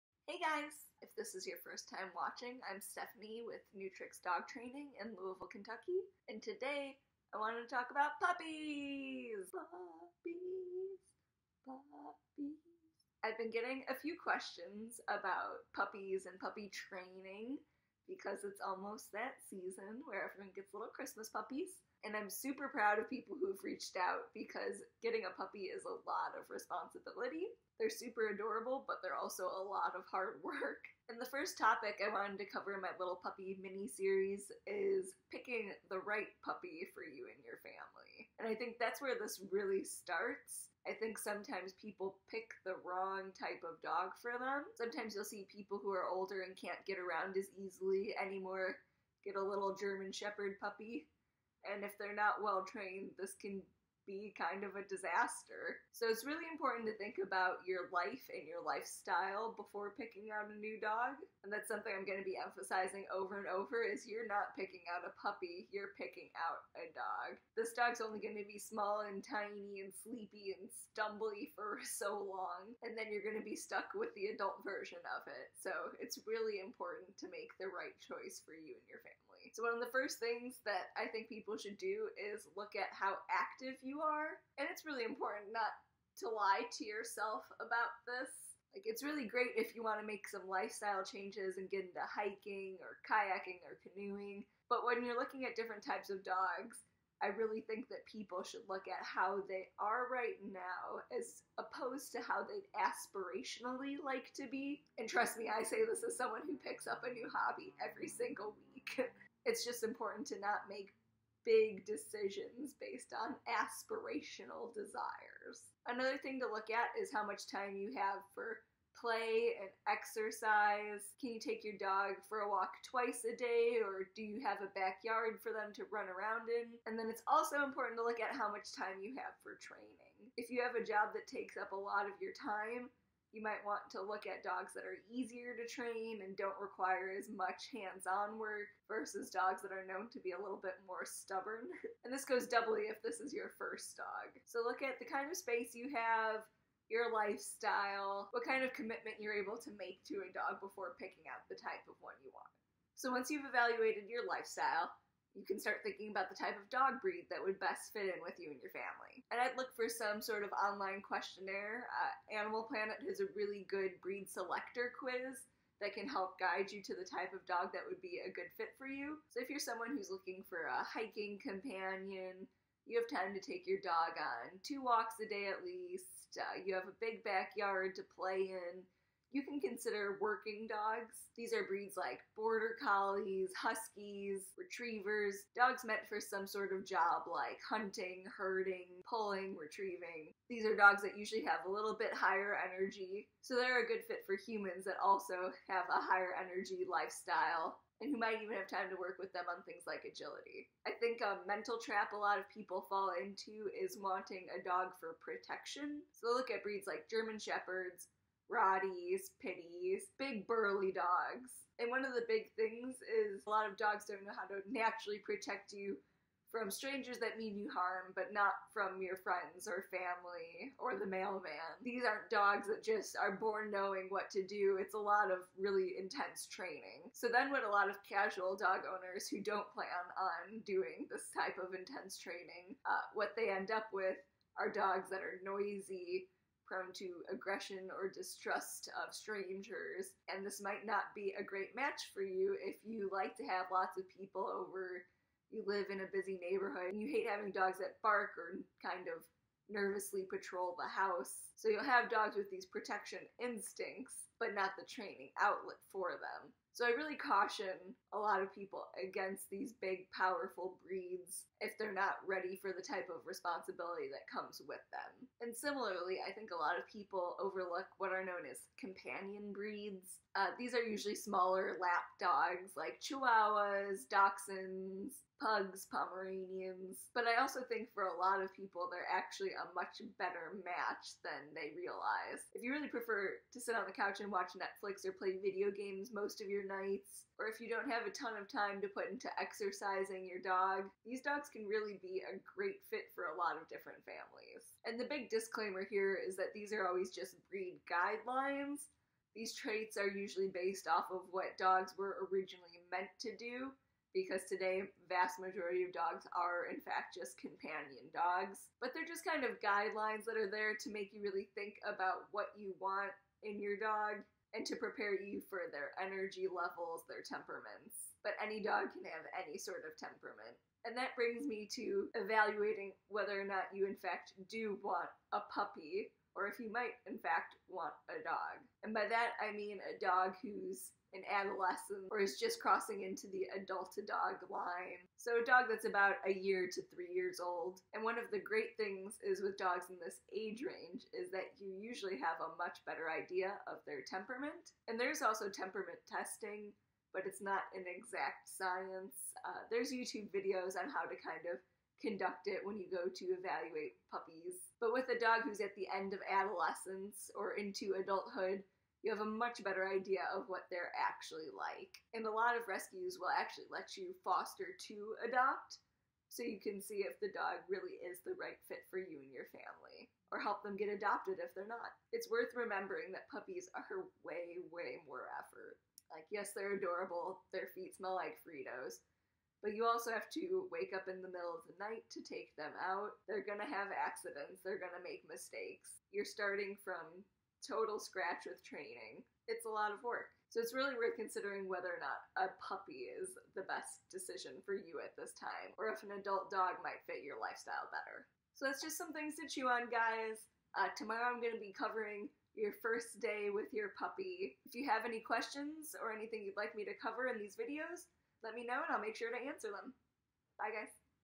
Puppy 101 : Picking a Puppy! (1/4)
In this first part of my Puppy 101 series, I'm taking a moment to talk about where it all begins - with the puppy you pick out!

The type of dog a family considers getting isn't always given that much consideration. After all, it's pretty easy to fall in love with any little fluffy football you find! But the puppy you pick out can GREATLY affect how happy you end up being with each other. Things like your activity level, the amount of time you have to train, the type of space you live in - these all influence what types of breeds/mixes that you would be best suited for! If you're not sure, make sure to check out a Breed Selector quiz to get some ideas and when in doubt talk to your local rescue volunteers or responsible breeder!

And remember - no matter what you're looking for in a dog, nothing is more important than some good solid training! Even dogs that are small couch potatoes can have a reliable off-leash recall and rock solid obedience!